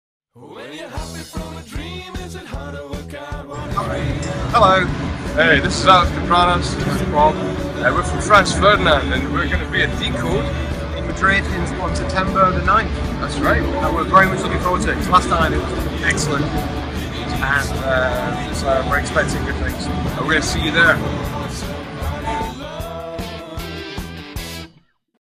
Saludo de Franz Ferdinand para DCODE
Alex Kapranos y Bob Harley de Franz Ferdinand te citan el 9 de septiembre en DCODE.